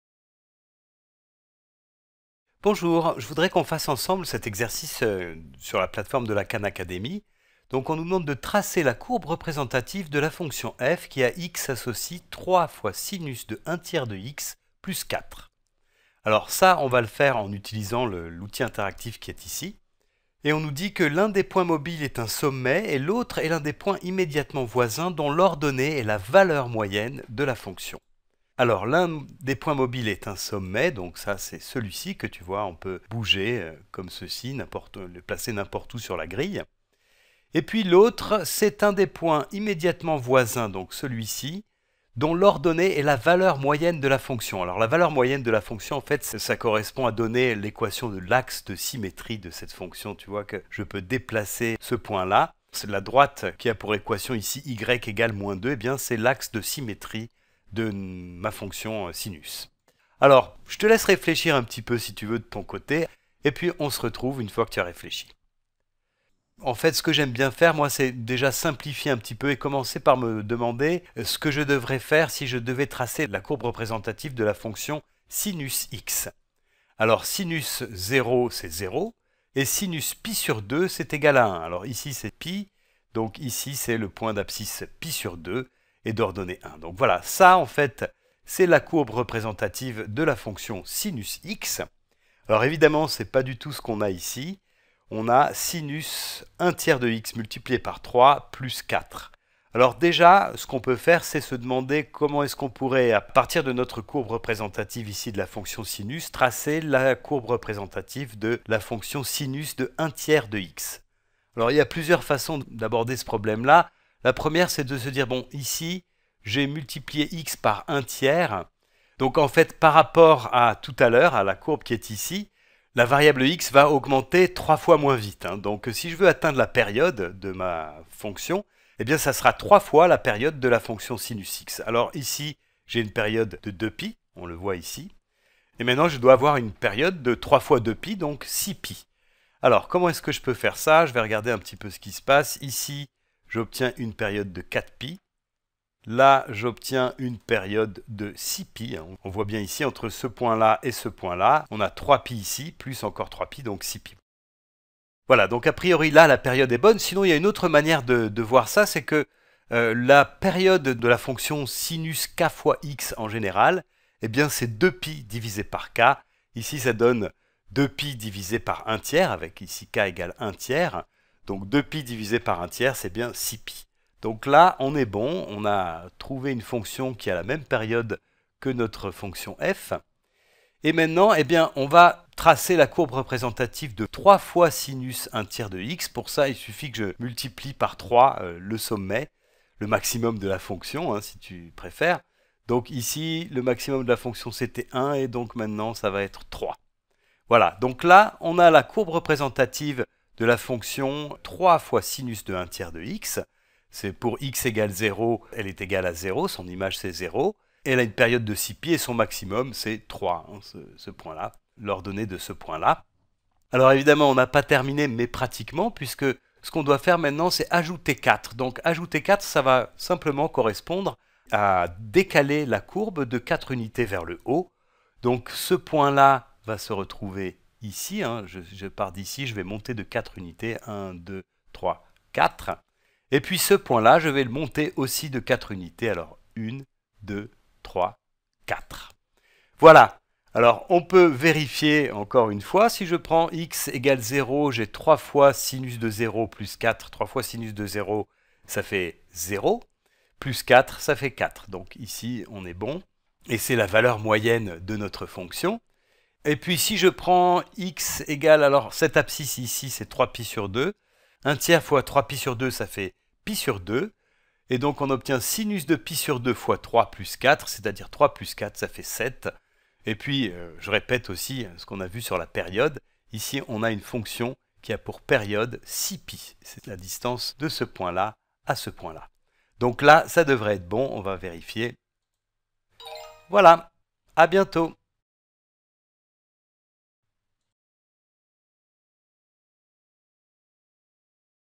Exemple - Tracer la courbe représentative de y=3⋅sin(1/3⋅x)+4
On trace la RG de la fonction y=3⋅sin(1/3⋅x)+4 à partir de la courbe de la fonction y=sin(x) et en analysant les effets sur cette courbe (axe de symétrie, période et amplitude) des transformations réalisées pour passer de la fonction  y=sin(x) à y=3⋅sin(1/3⋅x)+4.